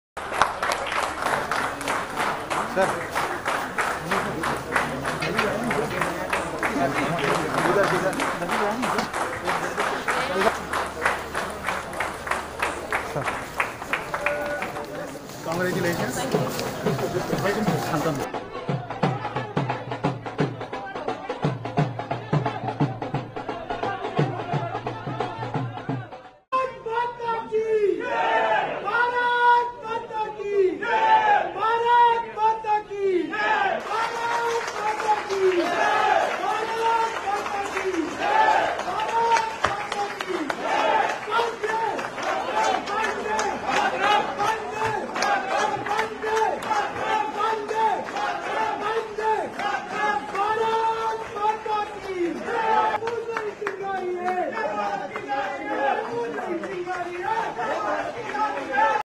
Proud Moments for India!!! | जब मेडल लेकर पी.वी. सिंघु और मीराबाई चानू देश पहुंची
Tokyo Olympic में medal लेना पूरे देश का सपना होता है। देश के 130 करोड़ लोगों  के लिए वो बहुत ही proud moment था जब bronze medal लेकर P.V. Sindhu airport पहुंची। Delhi Airport पर उनका धूमधाम तरीके से स्वागत किया गया।